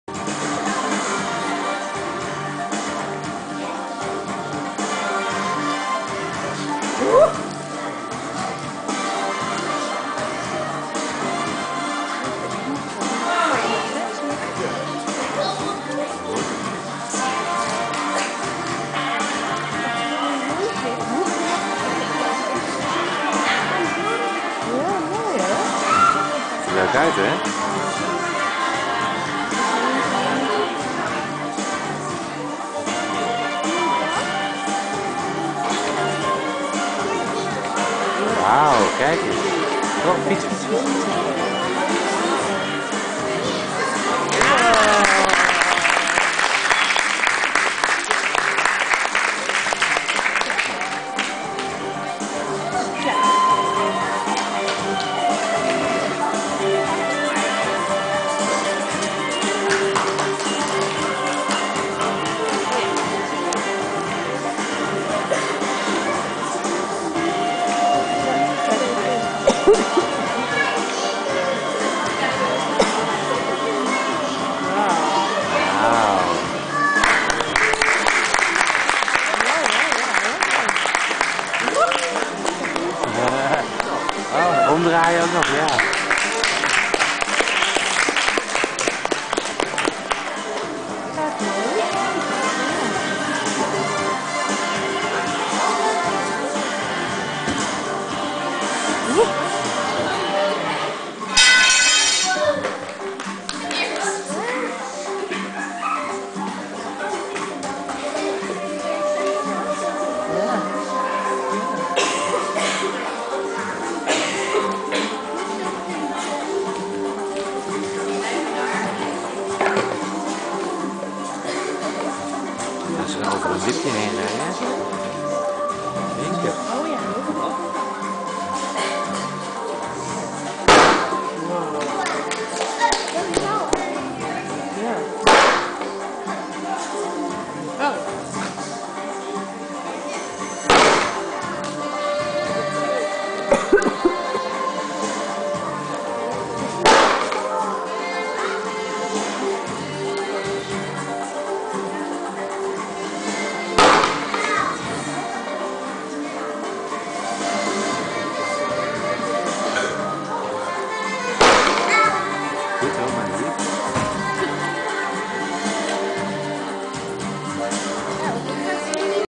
Pretpiste - Eénwieleract
Openbare repetitie voor de voorstelling in Het Huis aan de Poel door jeugdcircus Pretpiste in Amstelveen.